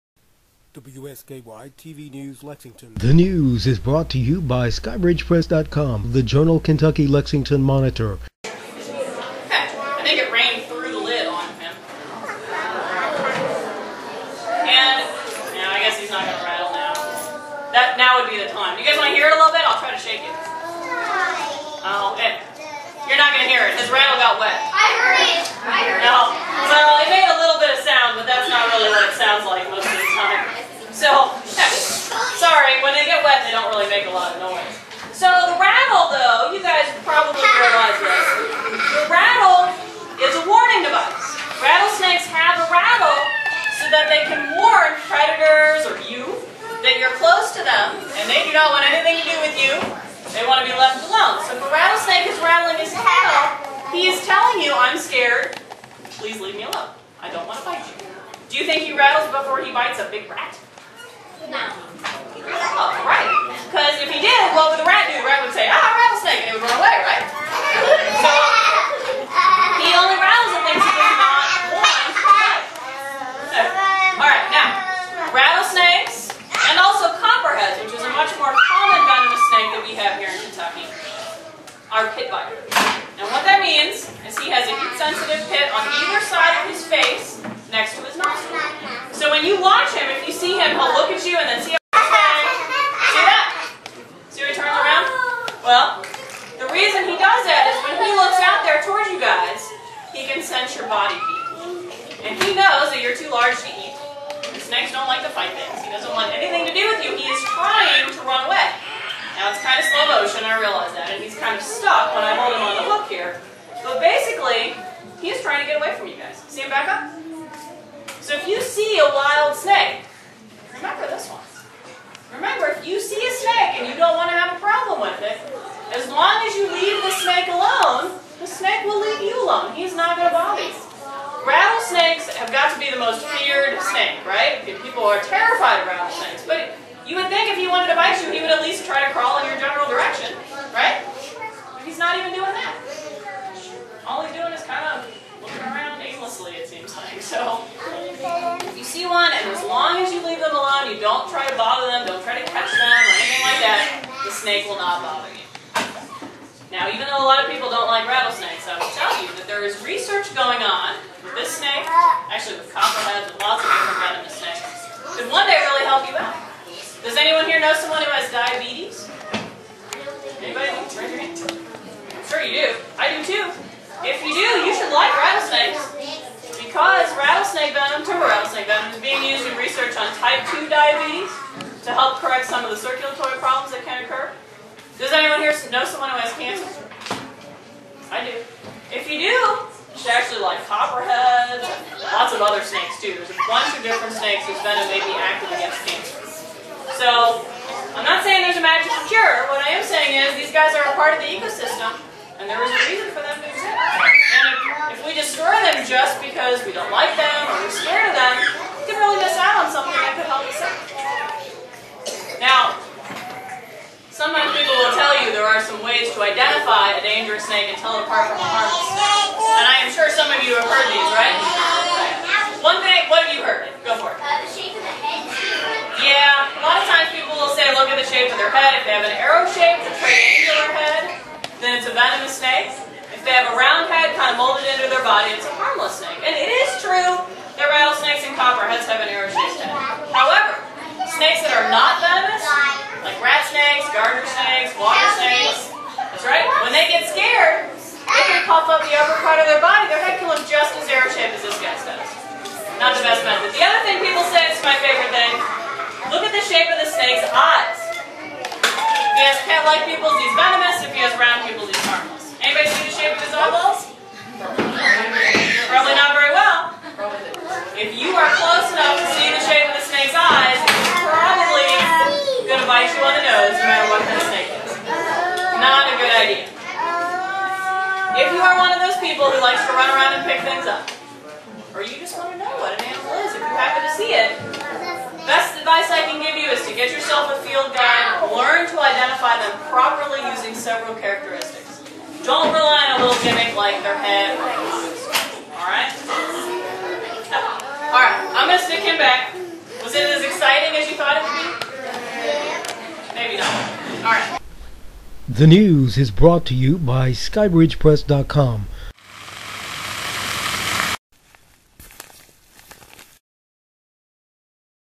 Part 2: Black Rat Snake
Kentucky Reptile Zoo representative lectures on the Black Rat Snake at the Living Arts and Science Center, Lexington, Kentucky.